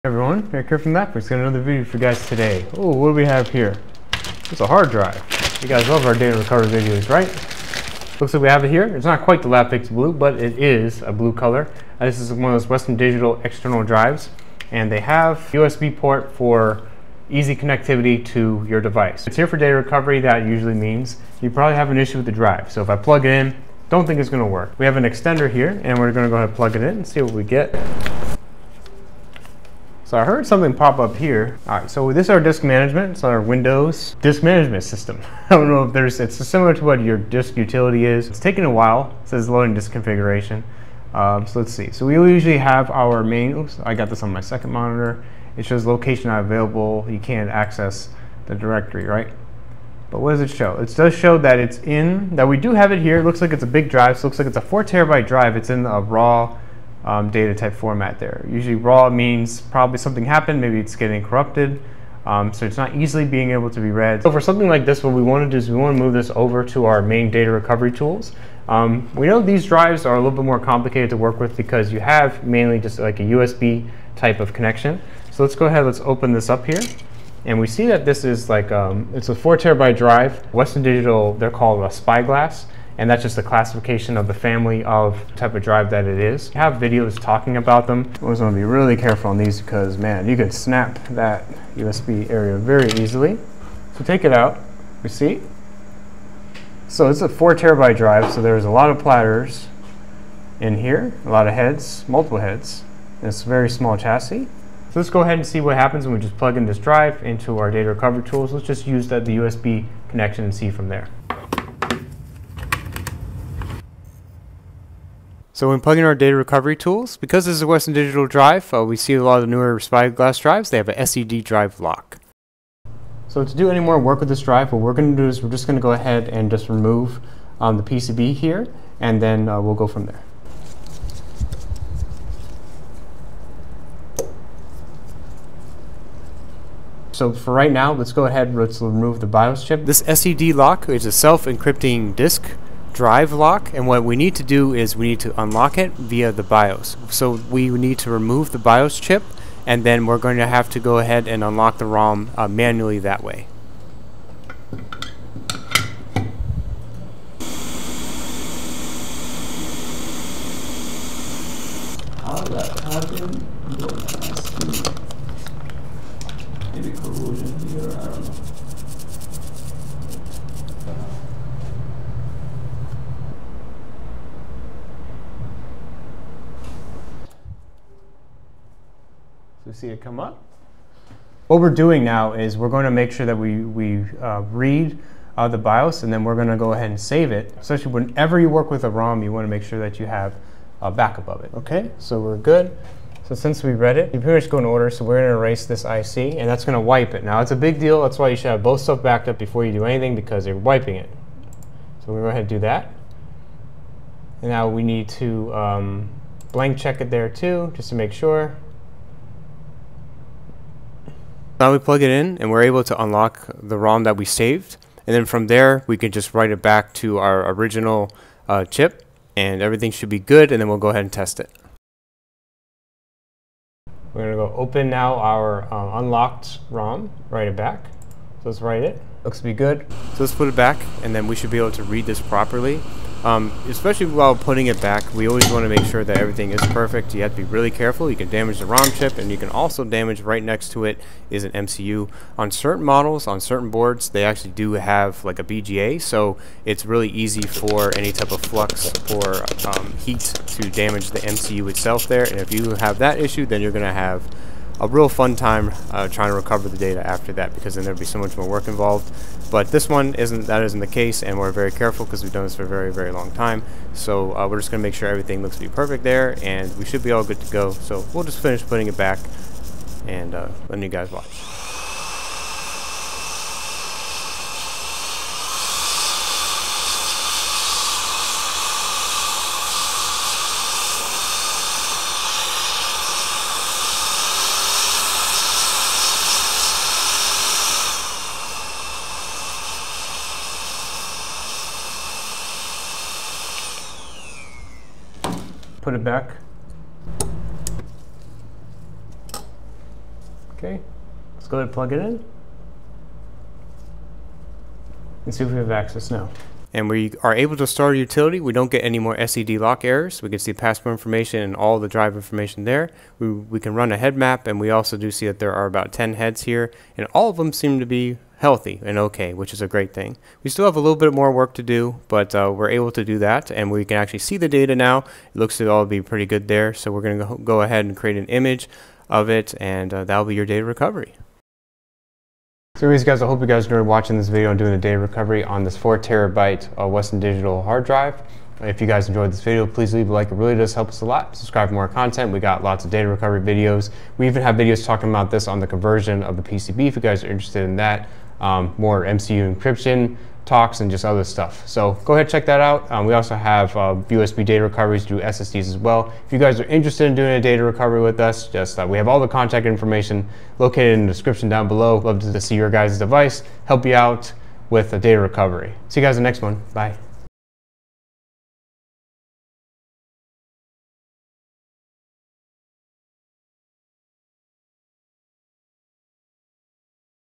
Recovering Data From An Encrypted 4TB WD External Hard Drive
Hello! Welcome back to the Channel!  Today we are going to be Recovering Data From An Encrypted 4TB WD External Hard Drive!